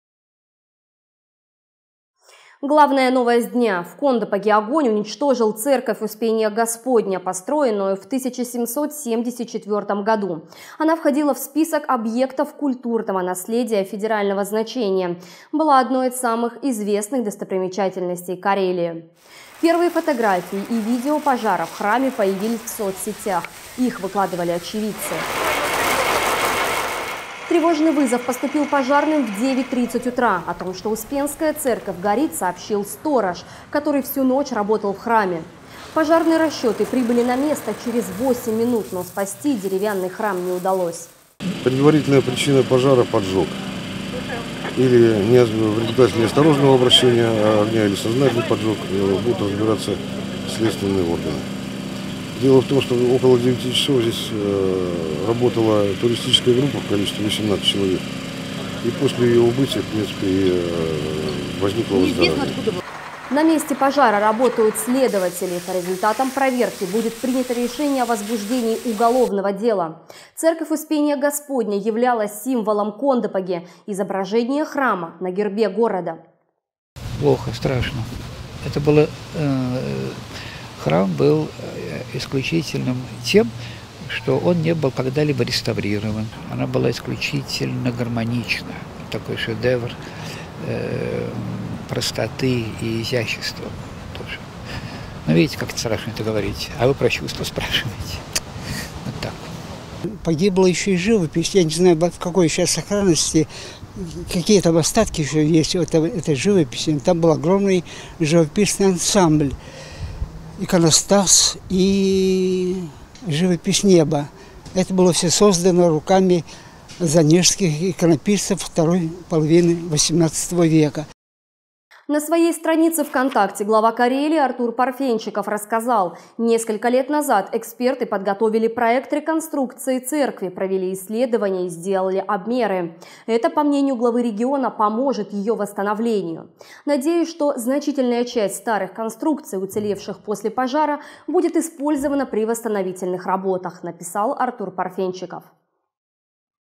В Кондопоге сгорела церковь Успения Господня
Предварительная причина возгорания - поджог. На месте пожара работают следователи. По результатам проверки будет принято решение о возбуждении уголовного дела.